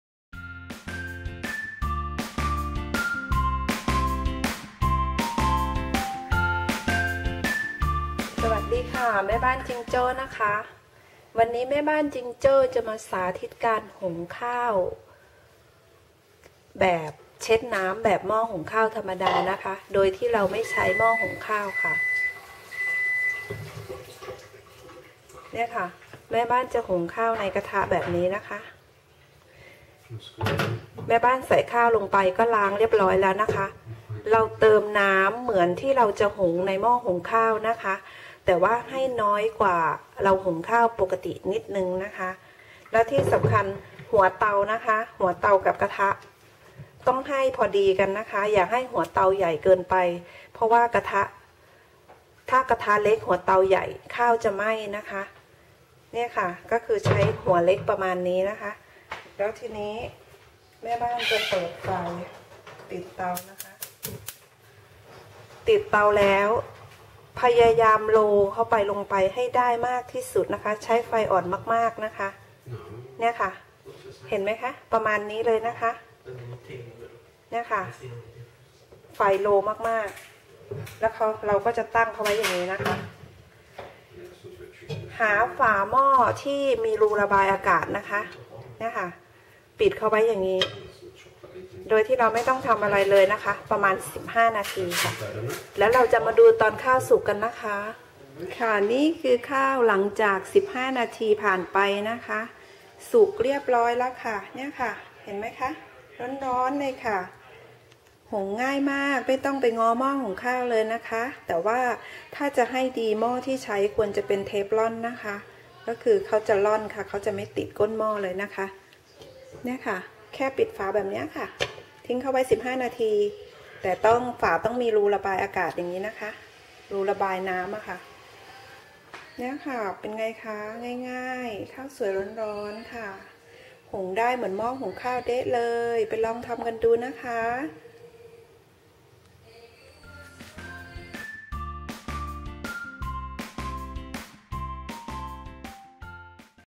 วิธีหุงข้าวโดยไม่ใข้หม้อ Easy way to cook rice
เกล็ดเล็กๆ น้อยๆ ที่ง่ายและสะดวกมากค่ะ บางทีเราก็เบื่อที่จะหุงข้าวในหม้อหุง กลับไปใช้วิธีแบบดั้งเดิม ก็สนุกดีนะคะ